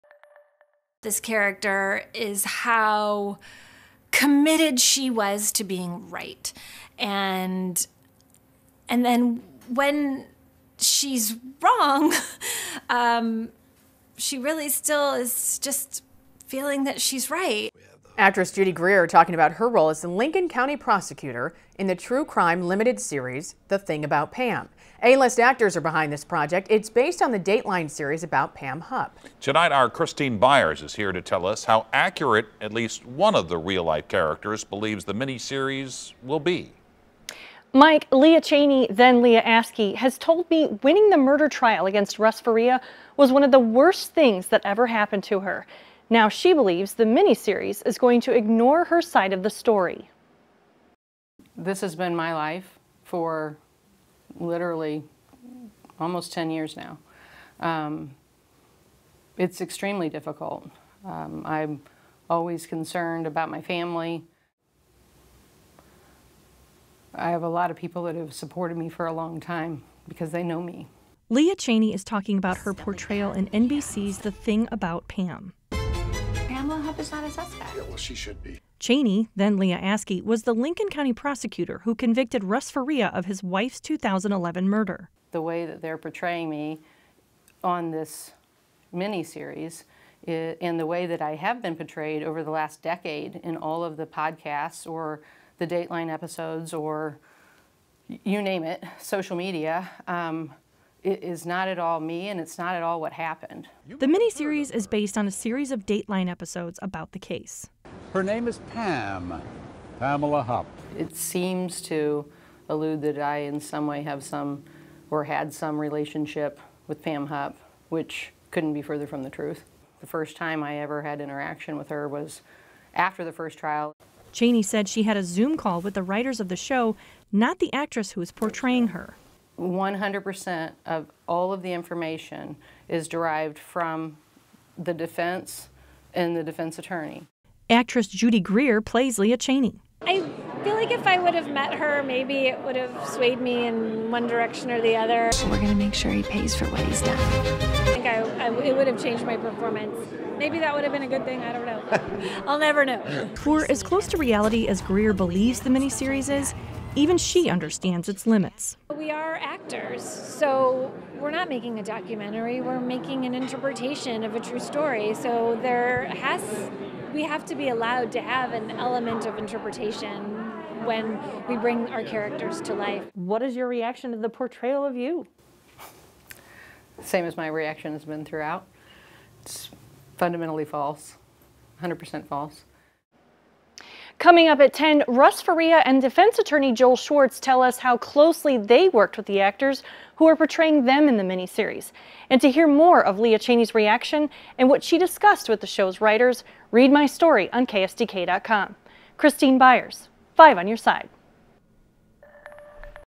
Prosecutor says 'The Thing About Pam' ignores her side of the story
Chaney said she has had virtually no input on "The Thing About Pam" miniseries.

“The way that they're portraying me on this miniseries is the way that I have been portrayed over the last decade in all of the podcasts or the Dateline episodes, social media, or you name it,” she said. “It’s not at all me, and it's not at all what happened.”

She continued: “It's fundamentally false, 100% false.”

Chaney said she’s only seen the trailers for the show so far.

“It seems to allude that I in some way have some or had some relationship with Pam Hupp, which couldn't be further from the truth,” she said. “The first time I ever had interaction with her was after the first trial.”

Chaney said Hupp came to her window at her office after Schwartz sent her a subpoena to testify during Russ Faria's second trial with the address to the former courthouse on it, and Hupp was confused.

Chaney said she refused to talk to Hupp without the conversation being recorded both audibly and visually, so they talked inside an interrogation room at the police department.

“I wanted to make sure that that conversation was 100% visible to the public, if ever necessary, because I know that she had made some allegations that people had swayed her one way or another or pressured her one way or another and I wanted to make sure that my conversation with her was completely transparent,” Chaney said.

Chaney said she saw pictures from that conversation used in a book Schwartz co-authored about the case called "Bone Deep."

She said she had one Zoom call with the writers of the miniseries.

“I wanted to make sure that if I were going to agree to the interview that at least they would listen to what I had to say and make sure that that was accurately portrayed,” Chaney said. “They asked me if I knew Pam Hupp and what my relationship was with Pam Hupp, and I explicitly told them the same thing that I just told you. I didn't have a relationship, I didn't know her before the first trial at all, I never even had spoken to her other than a pleasantry maybe before a deposition.”